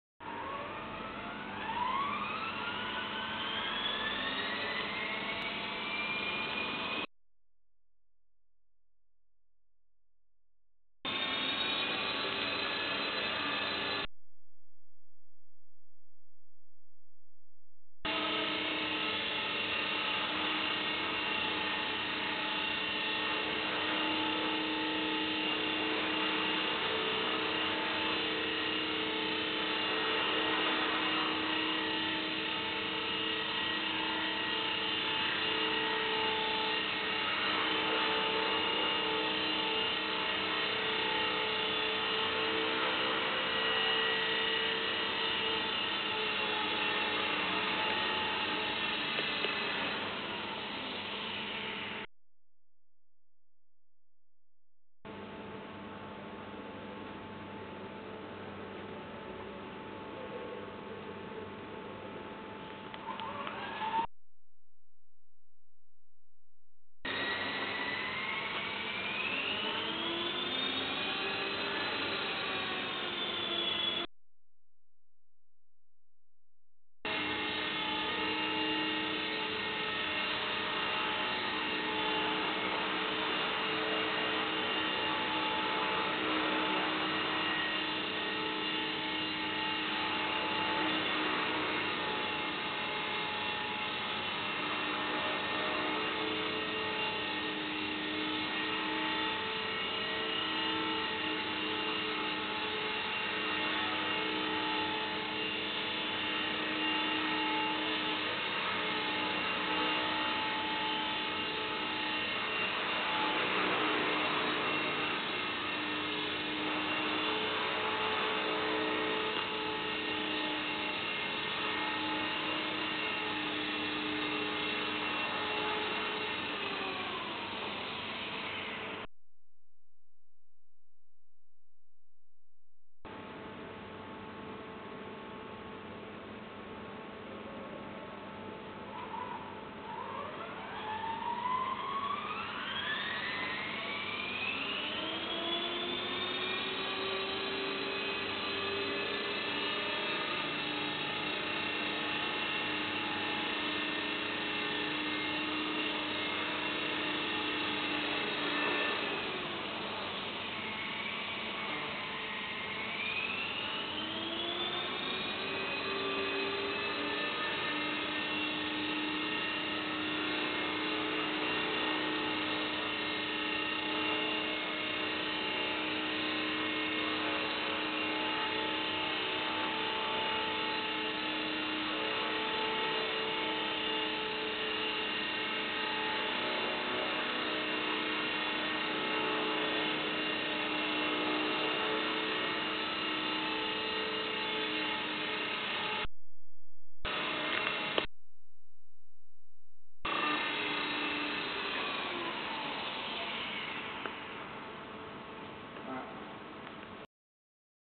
Mark's helicopter
The intrepid, and sternly concentrating RC pilot demonstrates his skills with the helicopter in closed quarters